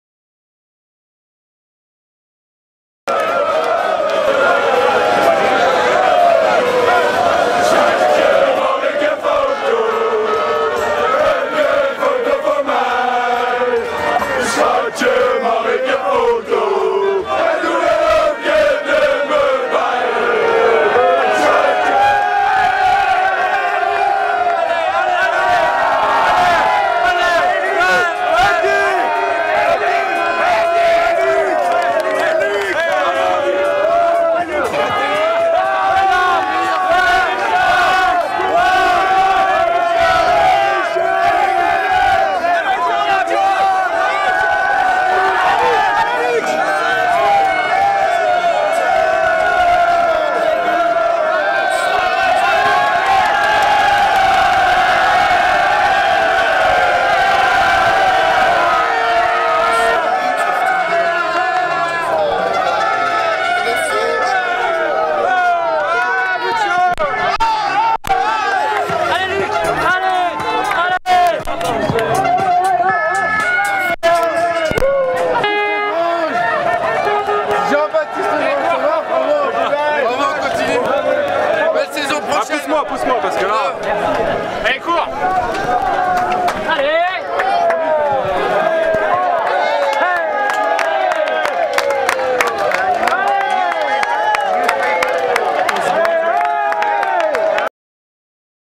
Alpe d'Huez - Tour de France 2015 - Luc Alphand, le retour
A quelques minutes du passage du peloton, Luc Alphand refait une montée de l'Alpe en compagnie de Jean Baptiste Grange